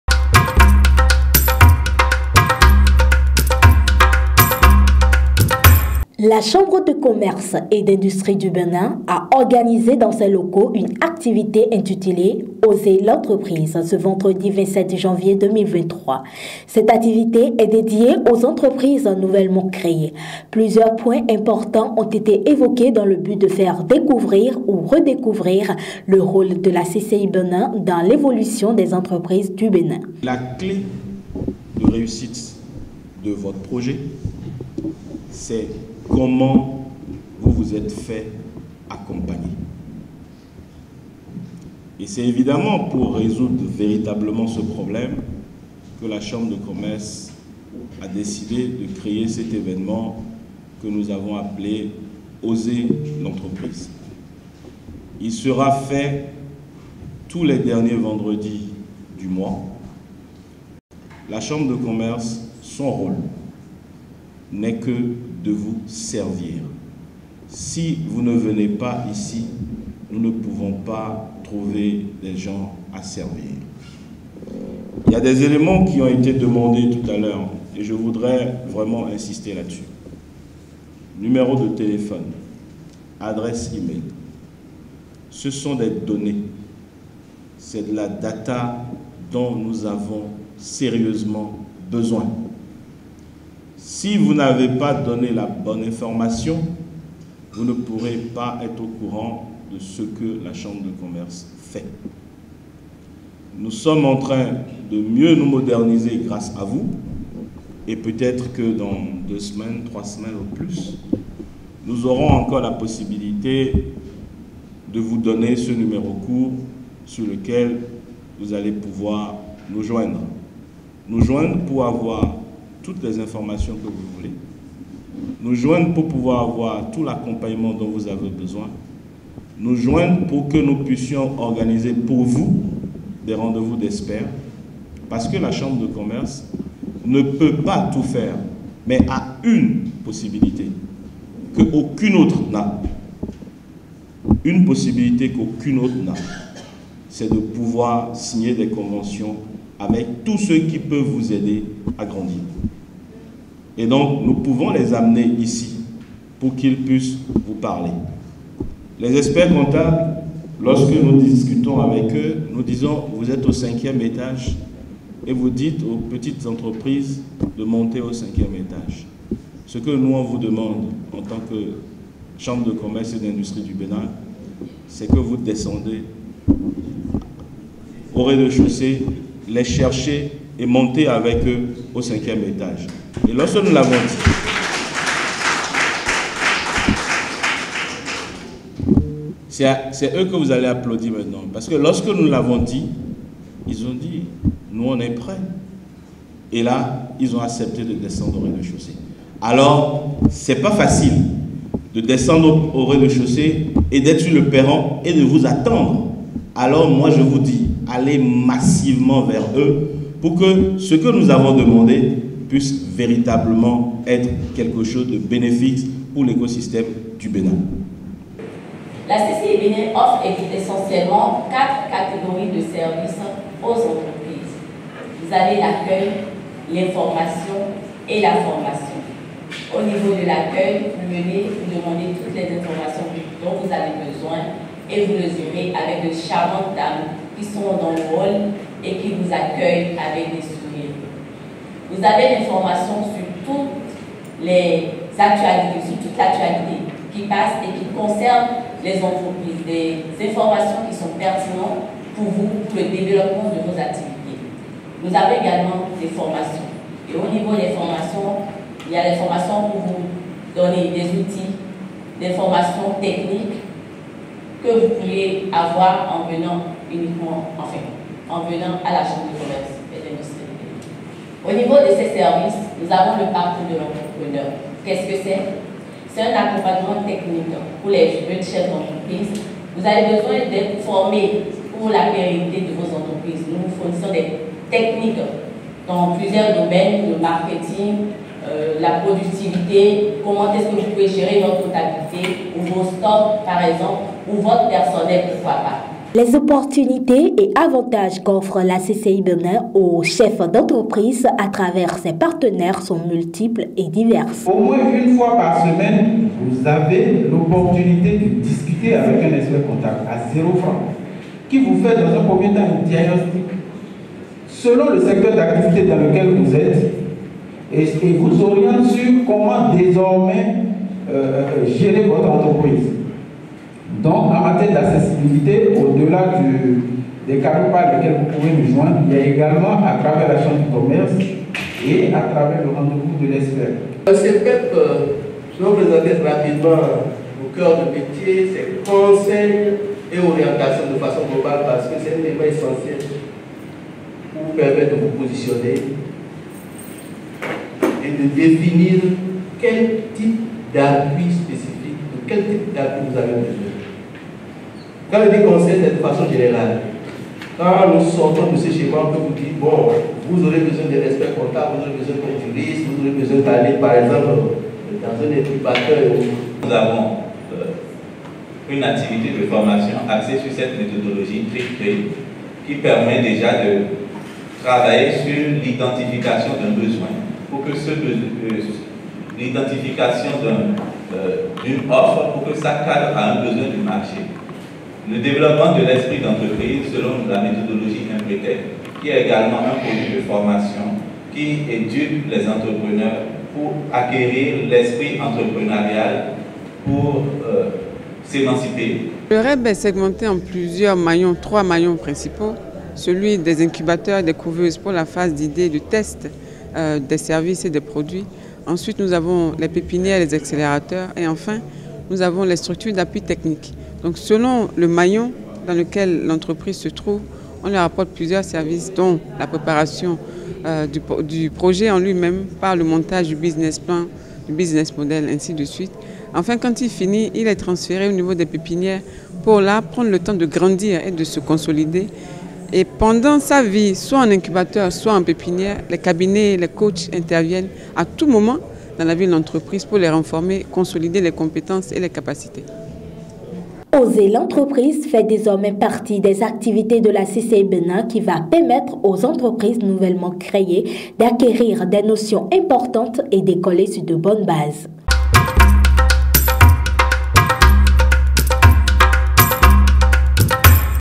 PROJET "OSEZ L'ENTREPRISE": LA CCI-BÉNIN OUTILLE LES CHEFS D'ENTREPRISES.
La Chambre de commerce et  d'industrie du Bénin a organisé dans ses locaux une activité intitulée OSEZ L'ENTREPRISE ce vendredi 27 Janvier 2023. Cette activité est dédiée aux entreprises nouvellement créées. Plusieurs points importants ont été évoqués dans le but de faire découvrir ou redécouvrir, le rôle de la CCI Bénin dans l'évolution des entreprises du Bénin.

Dans cette vidéo, vous aurez le compte rendu de KINGOTVWORLD de: PROJET "OSEZ L'ENTREPRISE": LA CCI-BÉNIN OUTILLE LES CHEFS D'ENTREPRISES.